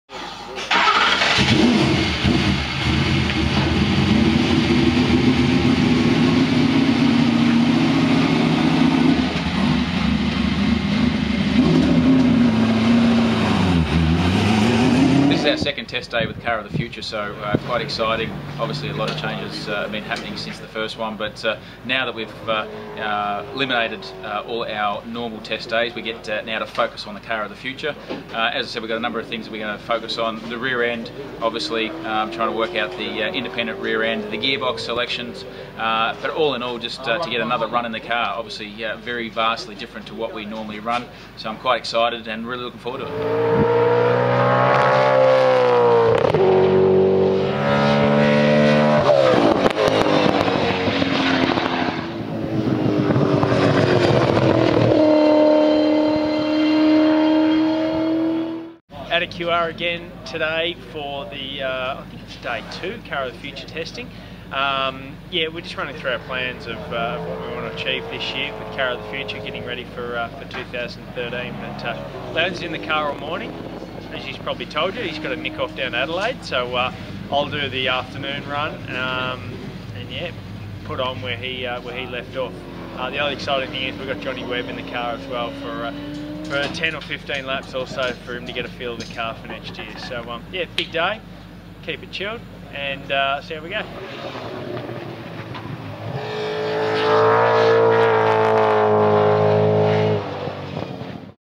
TeamVodafone COTF Test Day 2
TeamVodafone headed to Queensland Raceway today for a Test Day dedicated to the Car of the Future. Craig Lowndes started the day off followed by Jamie Whincup later in the afternoon.